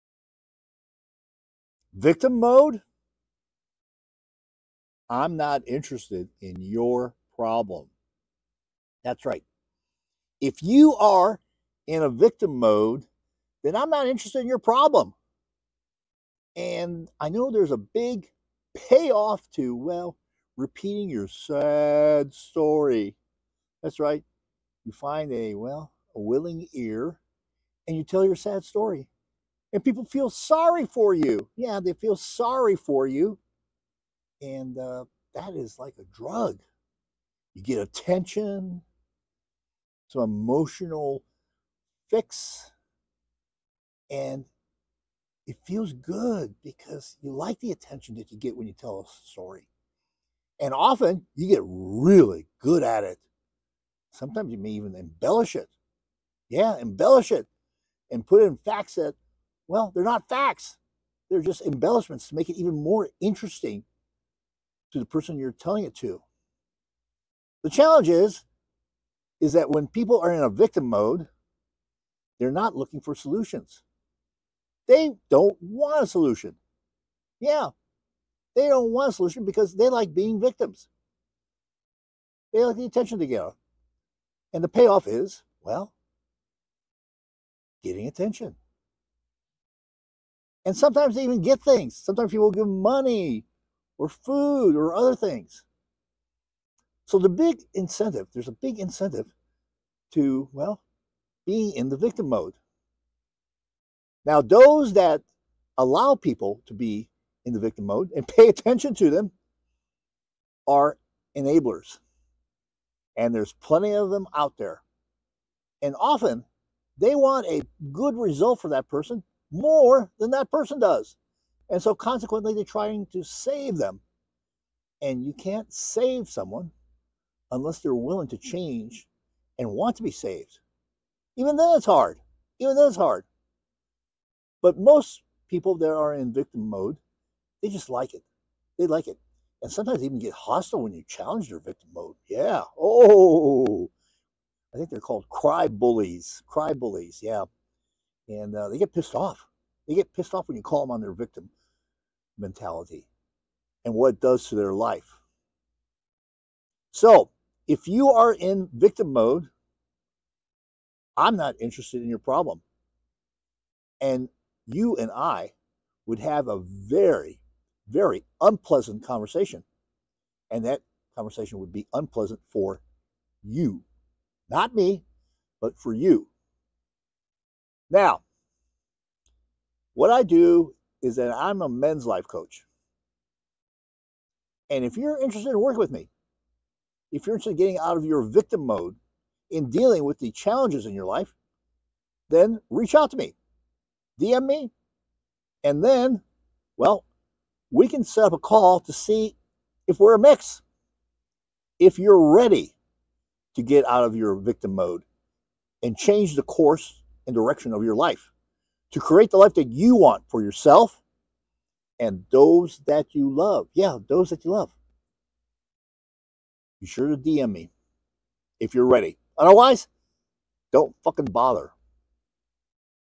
victim Mode I'm Not Interested In Your problem
That's right. If you are in a victim mode, then I'm not interested in your problem. And I know there's a big payoff to, well, repeating your sad story. That's right. You find a, well, a willing ear, and you tell your sad story, and people feel sorry for you.

Yeah, they feel sorry for you. And, that is like a drug. You get attention, some emotional fix. And it feels good because you like the attention that you get when you tell a story. And often you get really good at it.
Sometimes you may even embellish it. Yeah, embellish it and put in facts that, well, they're not facts, they're just embellishments to make it even more interesting to the person you're telling it to.

The challenge is that when people are in a victim mode, they're not looking for solutions. They don't want a solution. Yeah, they don't want a solution because they like being victims.
They like the attention together. And the payoff is, well, getting attention. And sometimes they even get things. Sometimes people give money, food, or other things.

So the big incentive. There's a big incentive to, well, be in the victim mode.
Now, those that allow people to be in the victim mode and pay attention to them are enablers, and there's plenty of them out there. And often they want a good result for that person more than that person does.

And so consequently, they're trying to save them. And you can't save someone unless they're willing to change and want to be saved. Even then, it's hard. Even though it's hard.
But most people that are in victim mode, they just like it. They like it and sometimes even get hostile when you challenge their victim mode.

Yeah, I think they're called Crybullies, Crybullies. Yeah. And, they get pissed off. They get pissed off when you call them on their victim mentality and what it does to their life.
So if you are in victim mode, I'm not interested in your problem, and you and I would have a very, very unpleasant conversation. And that conversation would be unpleasant for you, not me, but for you.
Now, what I do is that, I'm a men's life coach. And if you're interested in working with me, if you're want getting out of your victim mode in dealing with the challenges in your life, then reach out to me, DM me,

And then, well, we can set up a call to see if we're a mix. If you're ready to get out of your victim mode and change the course and direction of your life to create the life that you want for yourself and those that you love. Yeah, those that you love.

Be sure to DM me if you're ready.
Otherwise, Don't Fucking Bother